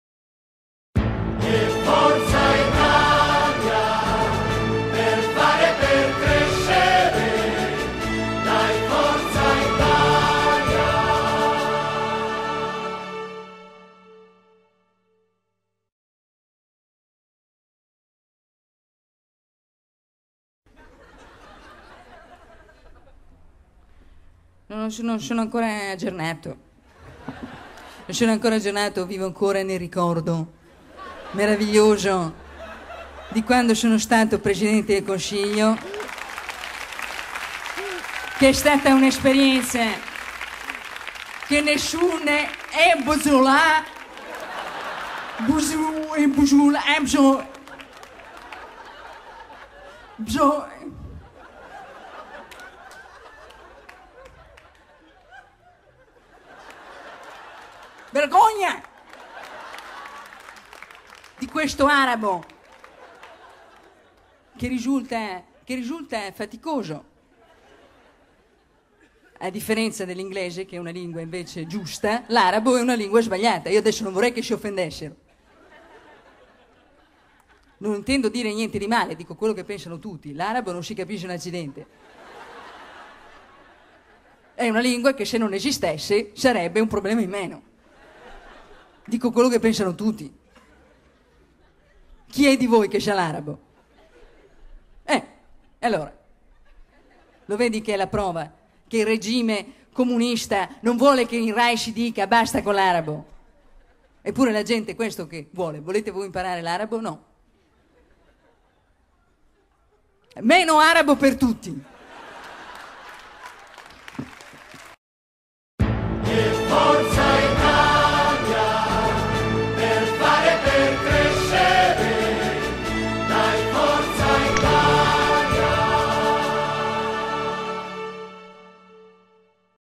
Sabina Guzzanti infiltrata speciale
La Guzzanti registra di nascosto il Cavaliere per imitarlo meglio